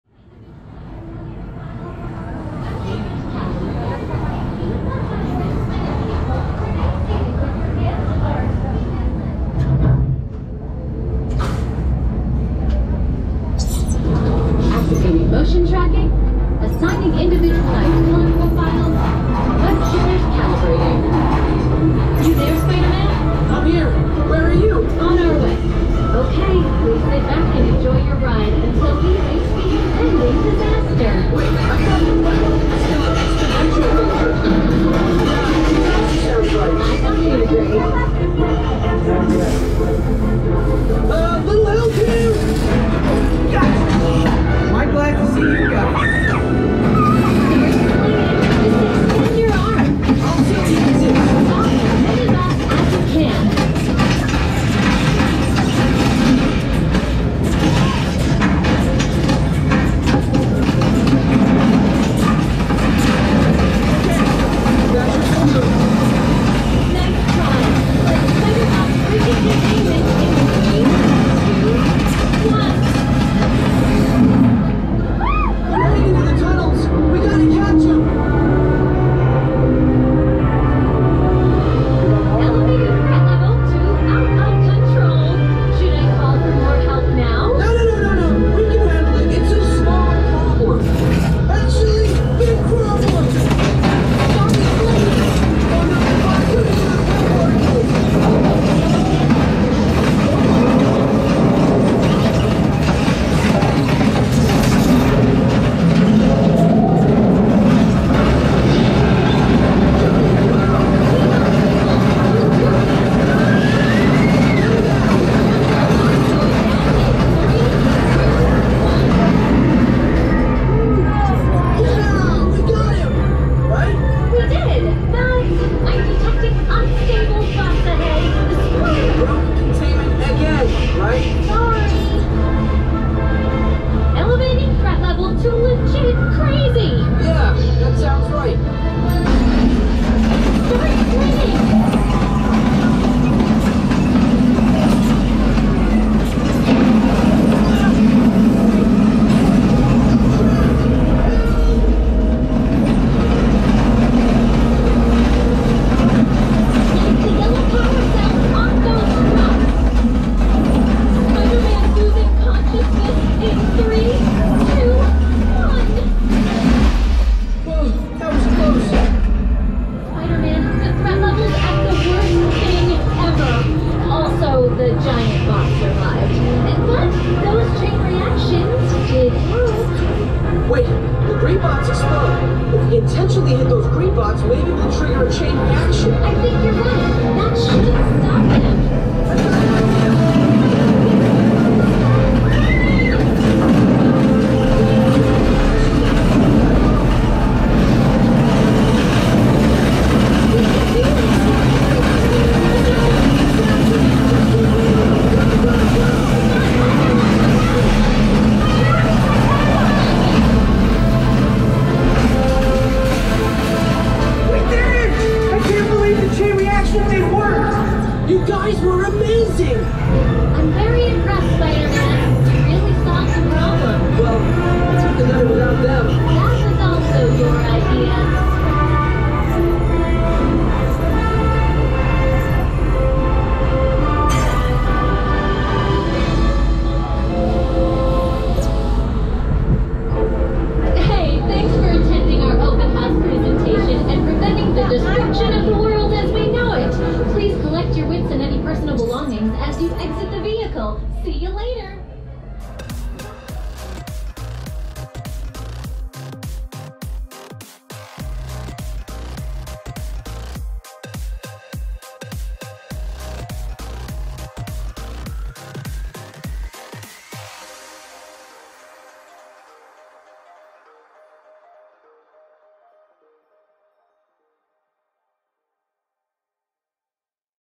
WEB SLINGERS: A Spider-Man Adventure Disneyland - Disney Parks Ride POV 4K60FPS
Get the best On-Ride POV of WEBSLINGERS located at Disney’s California Adventure!!